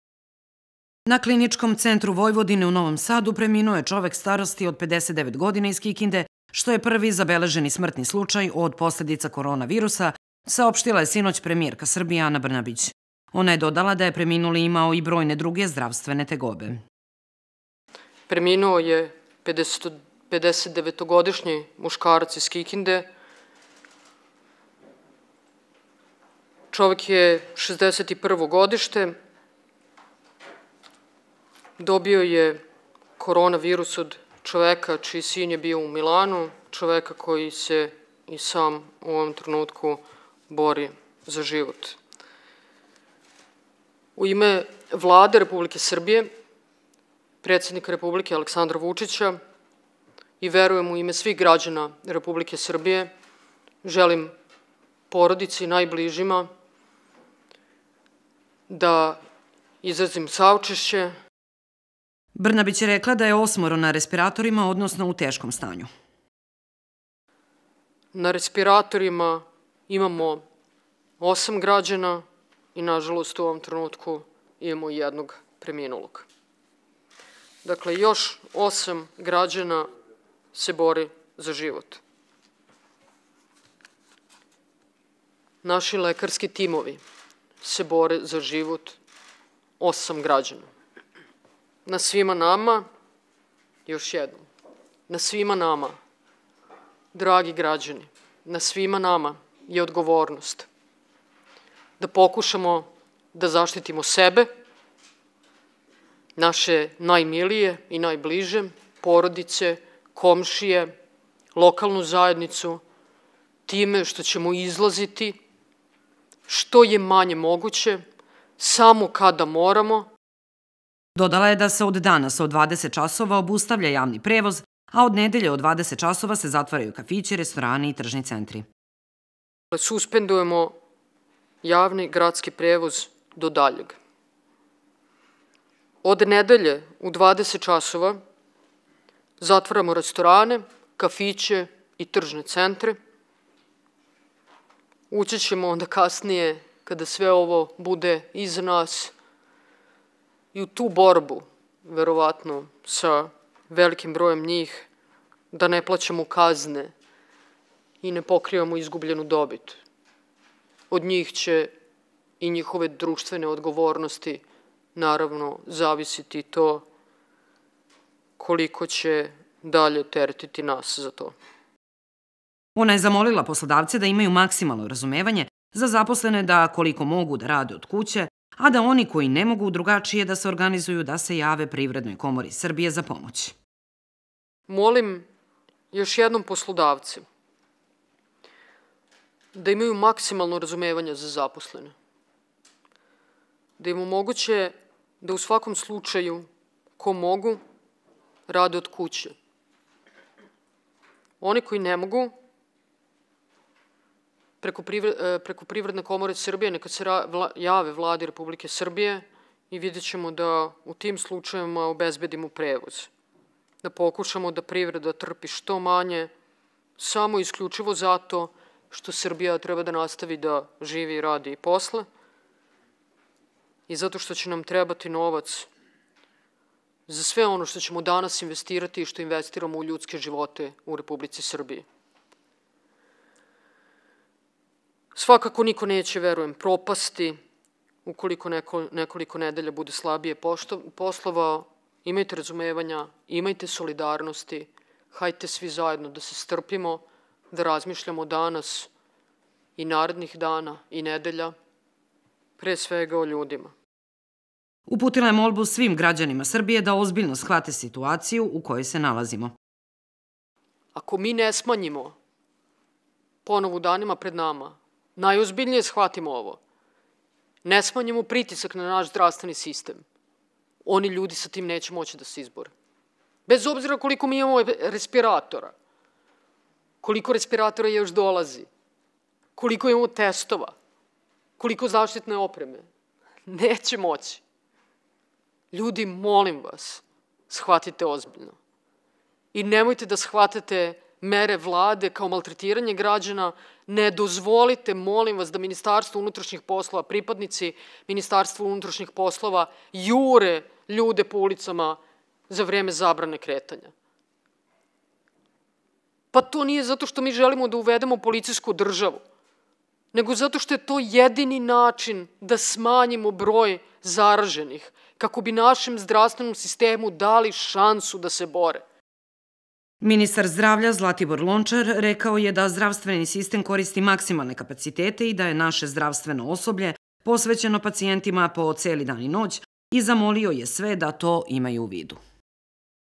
21 03 2020 PRVI SMRTNI SLUČAJ ZBOG POSLEDICA KORONA VIRUSA U SRBIJI
Muškarac iz Kikinde star 59 godina preminuo je u petak, čime je postao prva žrtva korona virusa u Srbiji. Kako je saopštila premijerka Ana Brnabić, još osam pacijenata se bori za život. „Pre nekoliko sati preminuo je 59-godišnjak iz Kikinde koji je dobio virus od čoveka čiji je sin bio u Milanu, a koji se sada i sam bori za život", kazala je premijerka i dodala da je čovek koji je preminuo imao i drugih zdravstvenih tegoba.